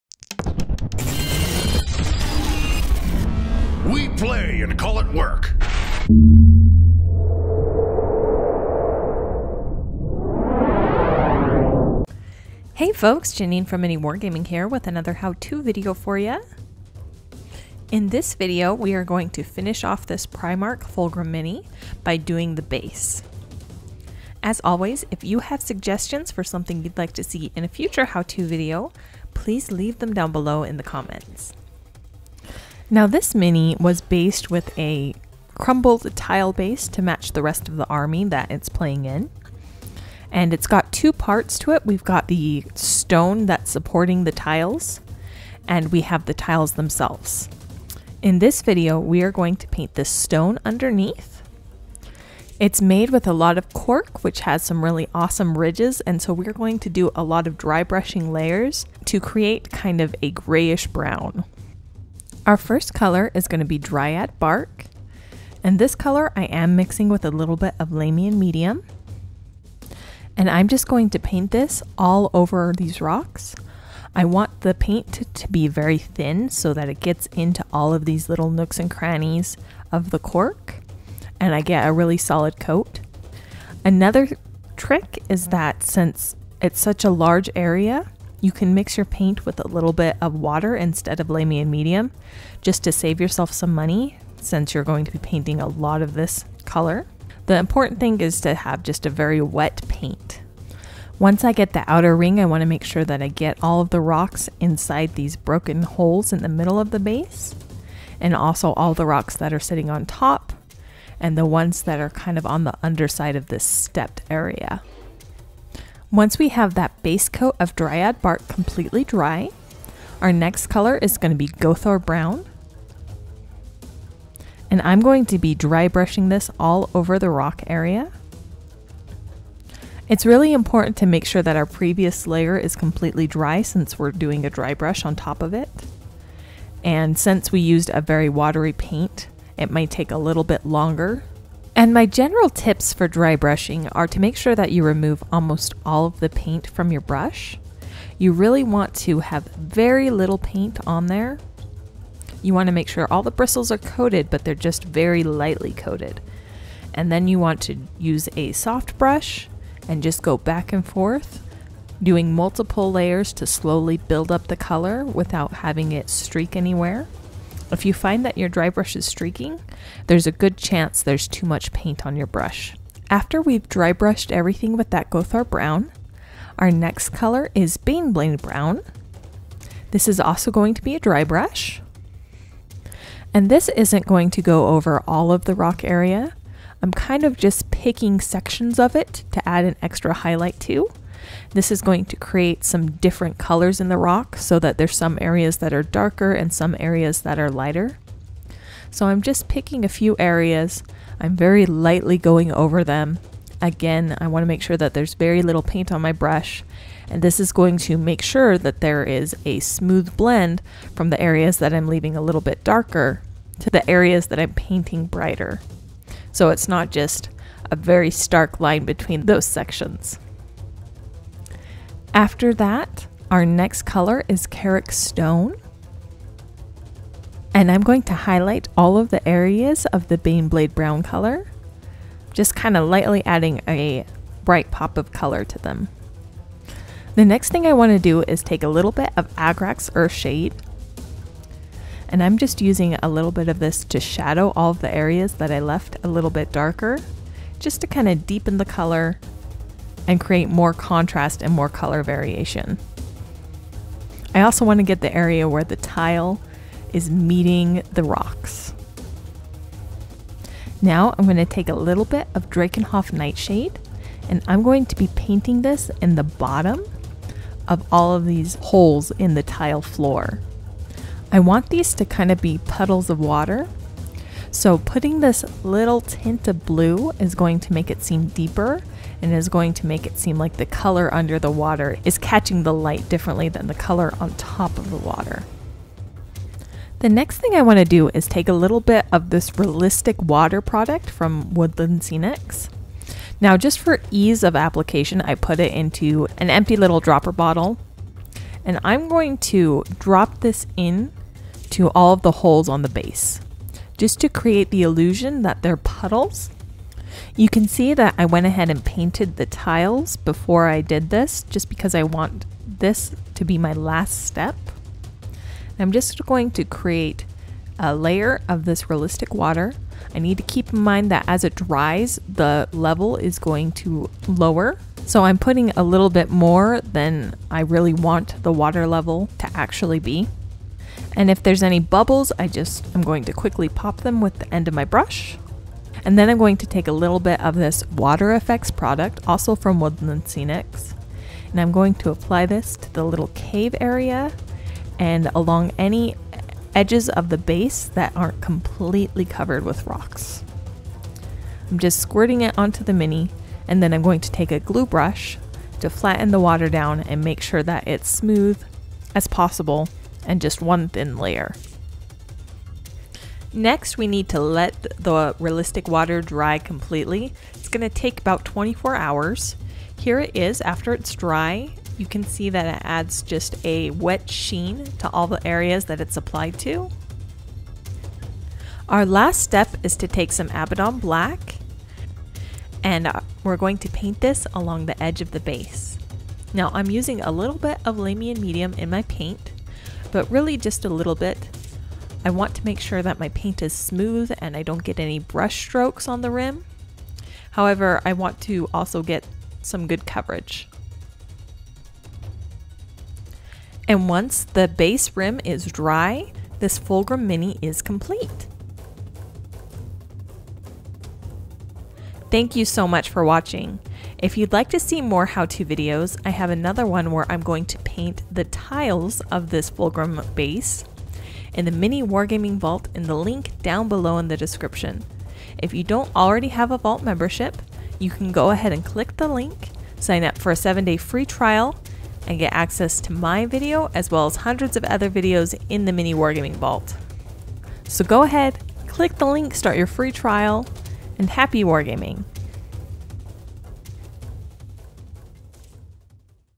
How to: Paint Brown Rocks with Puddles
In this video Janene shows you how to paint the brown rock pieces of Fulgrim's base with some water elements.